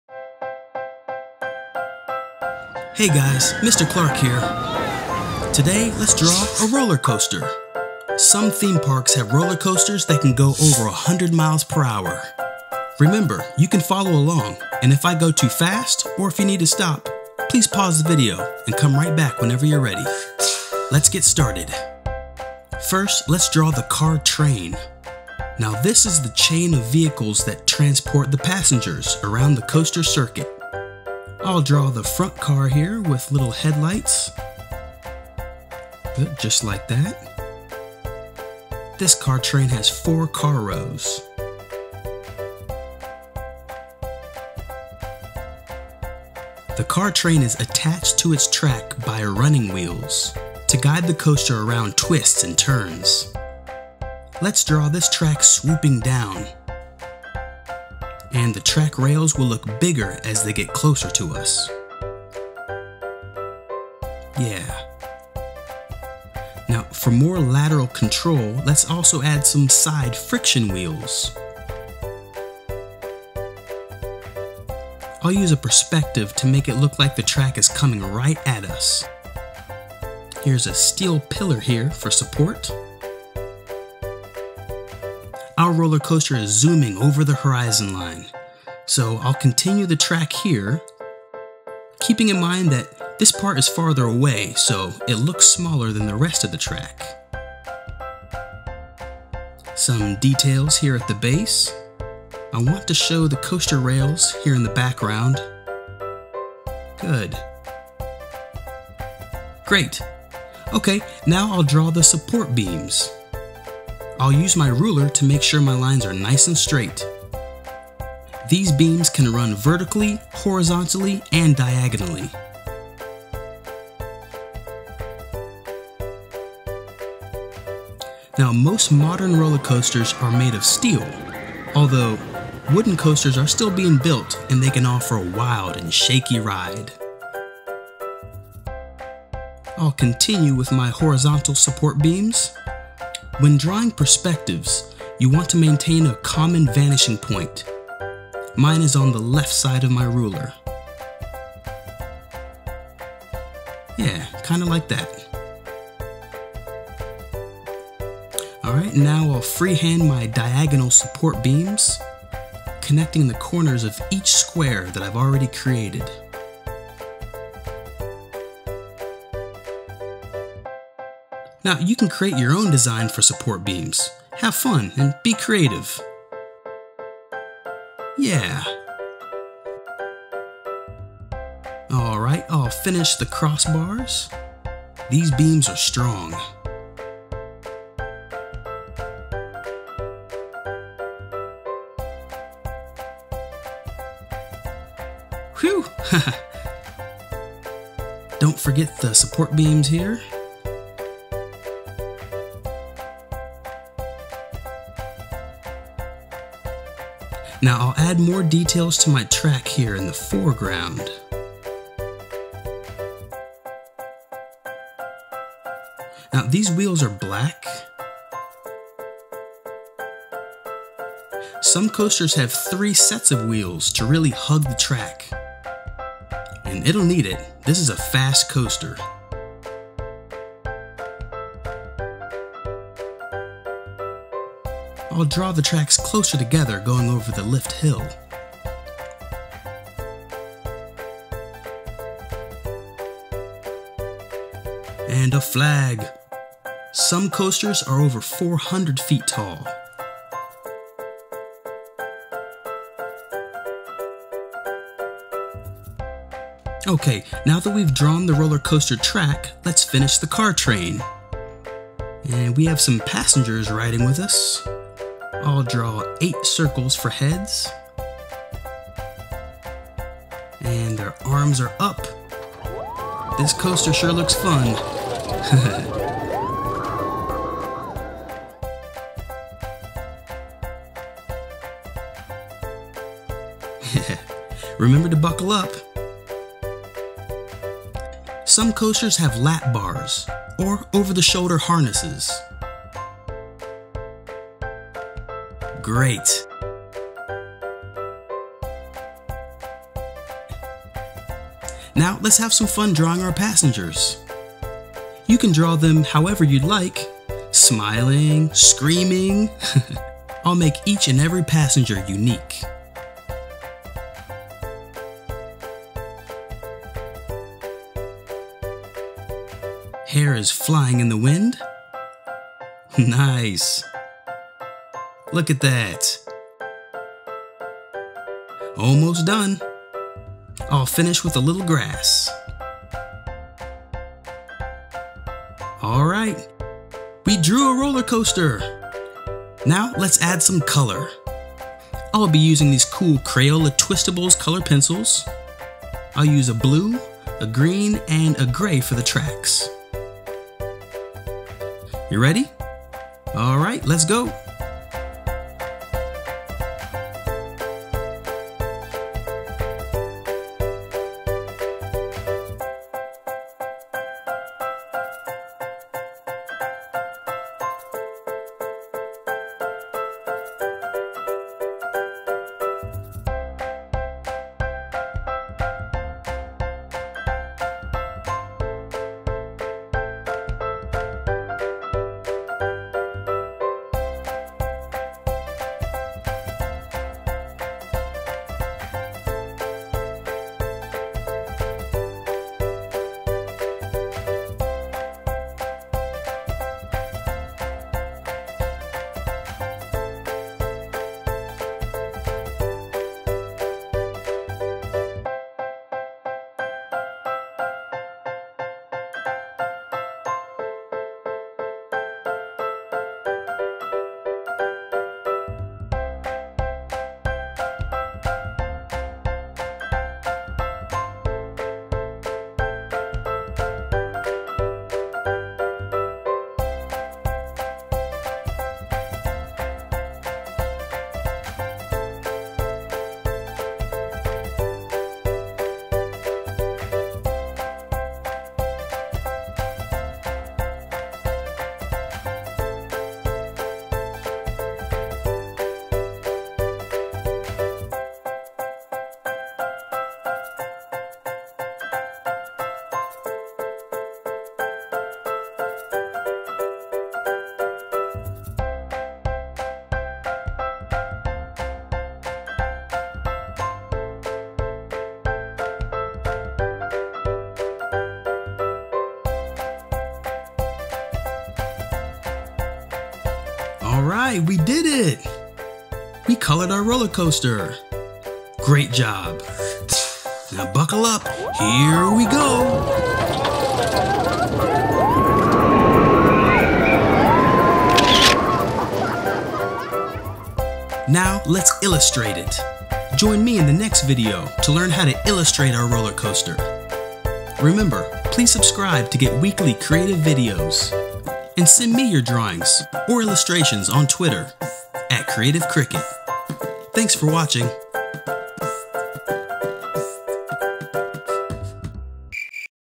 How to Draw a Roller Coaster Step by Step in One point Perspective
Learn how to draw a Roller Coaster design in this easy step by step drawing tutorial for kids! In this art lesson, we use a ruler to create a one point perspective design with a vanishing point. Have fun drawing funny reactions on your peoples faces!

Amusement parks often compete to build the tallest, fastest, and longest rides to attract thrill seekers and boost overall park attendance. There are various types of roller coasters including:  sit-down, stand-up, inverted, suspended, pipeline, bobsled, flying and even fourth dimension! Use some ideas you learn here to build your own roller coaster in Minecraft or using Legos! Be Creative, and make it your own!